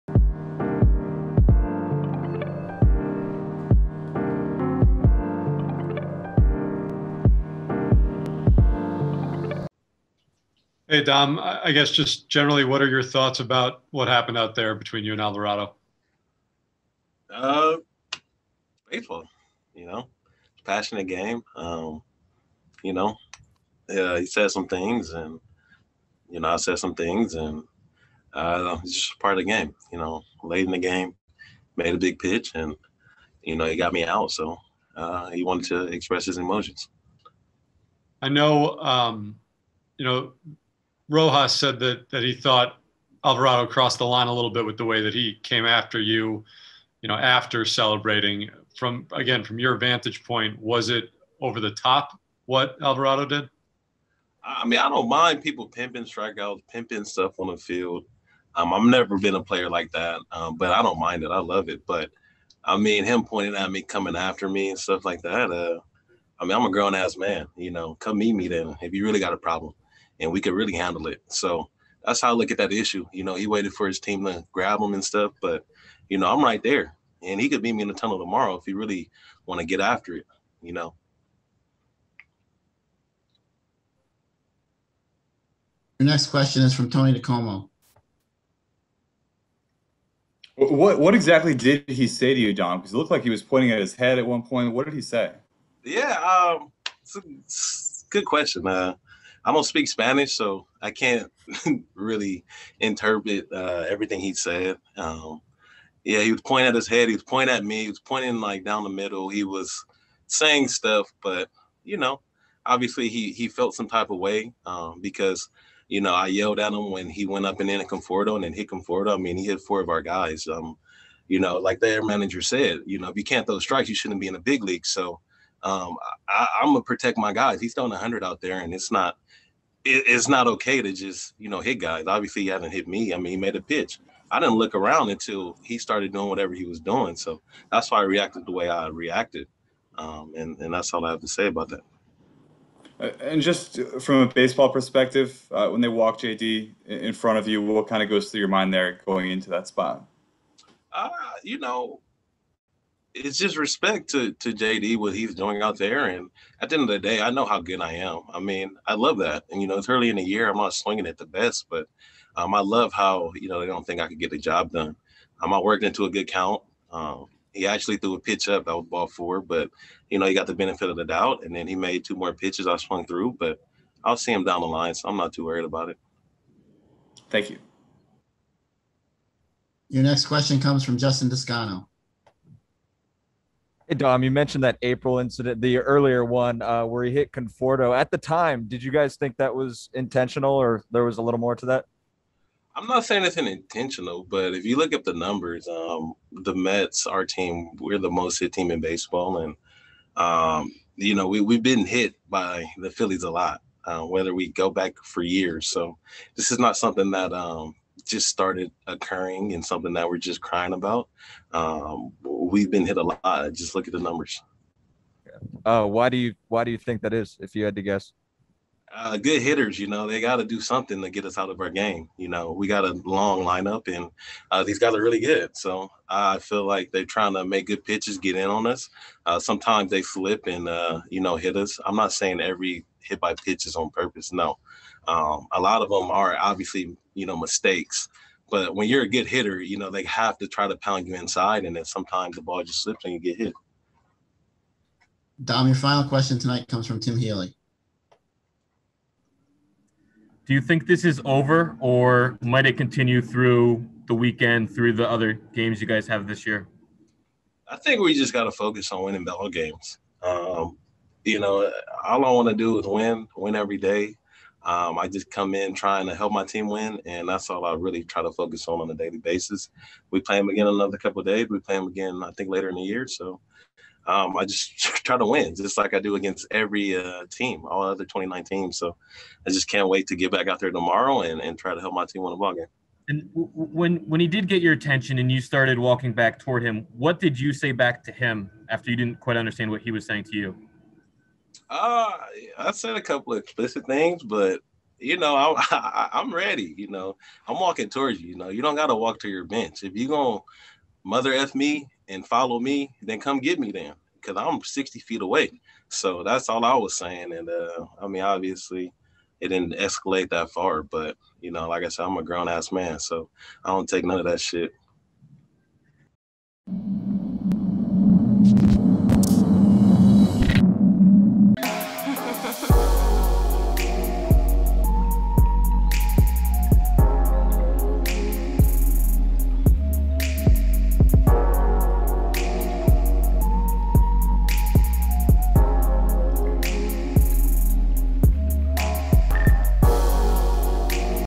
Dominic Smith Rips Jose Alvarado: "I'm a Grown A** Man, Come Meet Me. We Can Meet in the Tunnel"
Watch as Dominic Smith Rips Jose Alvarado: "I'm a Grown A** Man, Come Meet Me. We Can Meet in the Tunnel"